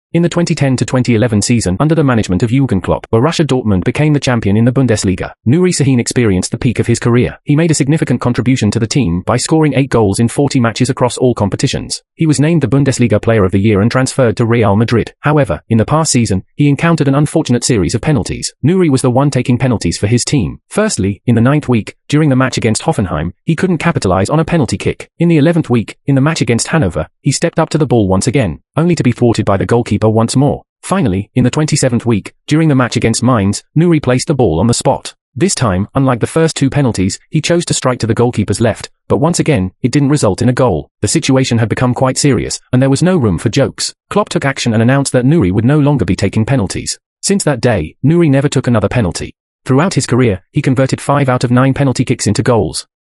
The season Nuri Sahin put an end to taking penalties
Nuri Şahin, one of Borussia Dortmund's most valuable players in the 2010/11 season, which culminated in clinching the Bundesliga championship by shattering Bayern Munich, had a rather poor streak in penalties.